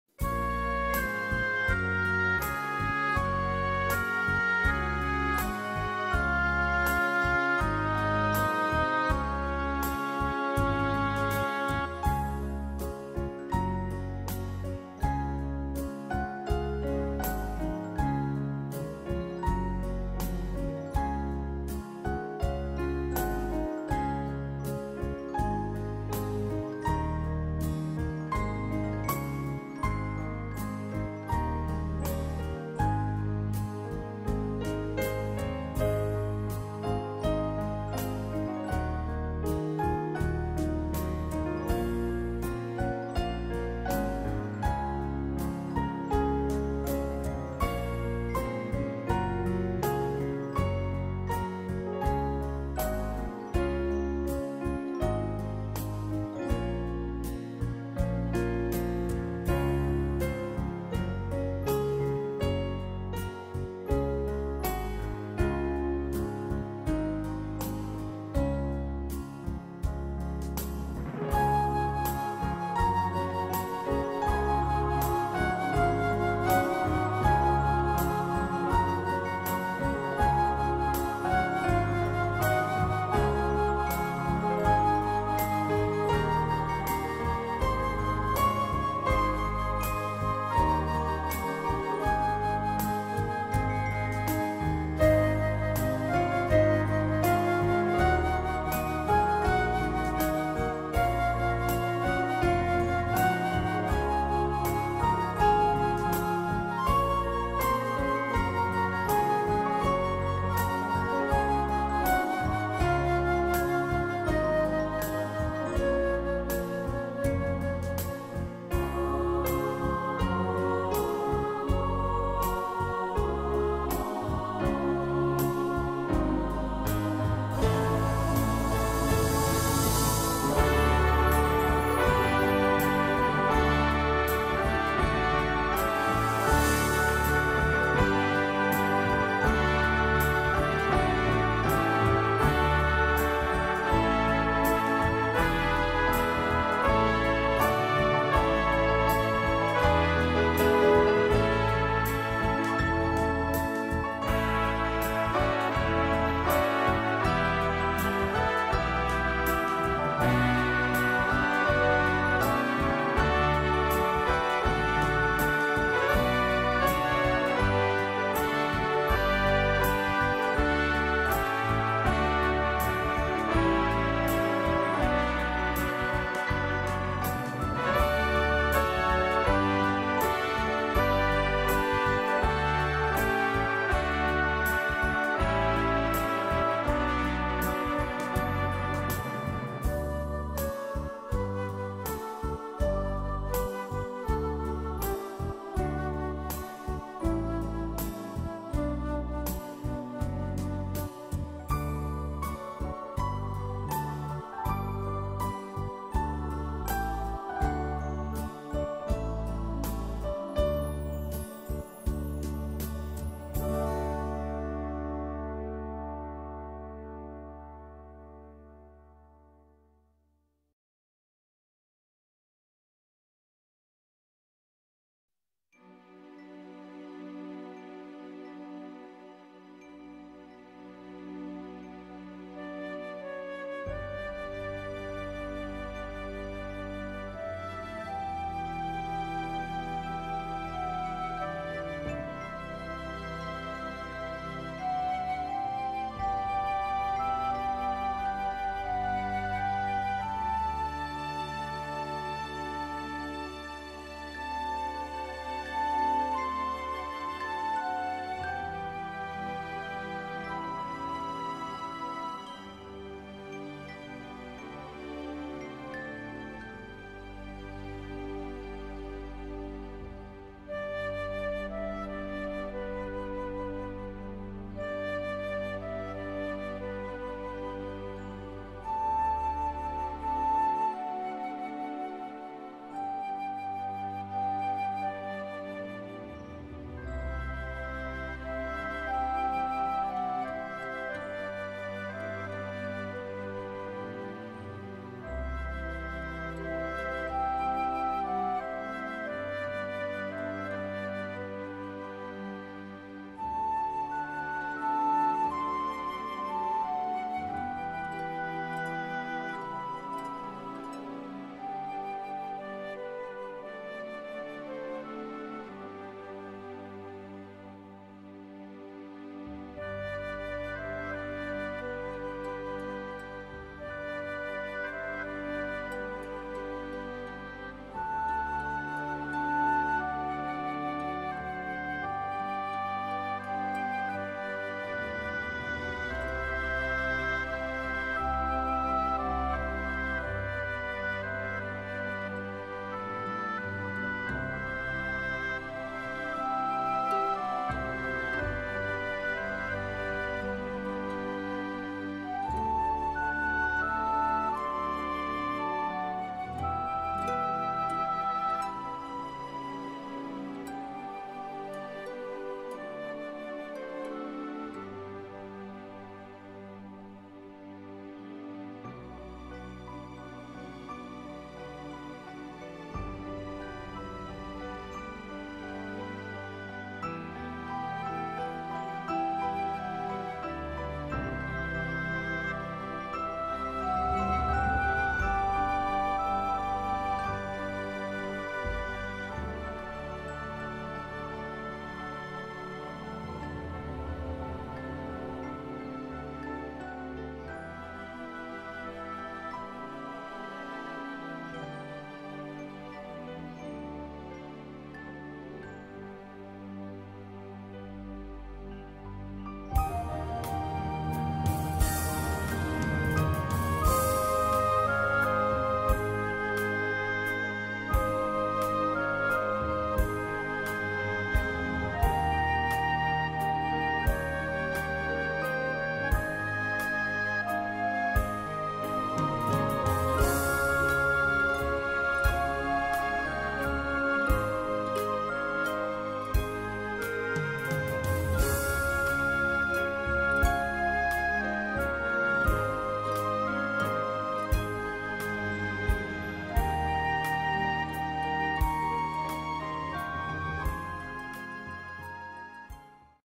Trierer Weihnachtsmarkt 2023
Der traditionelle Gang über den Trierer Weihnachtsmarkt, dieses Jahr etwas kürzer, da wir bereits in den vergangenen Jahren sehr viel Videomaterial darüber hochgeladen haben. Musik Ronny Matthes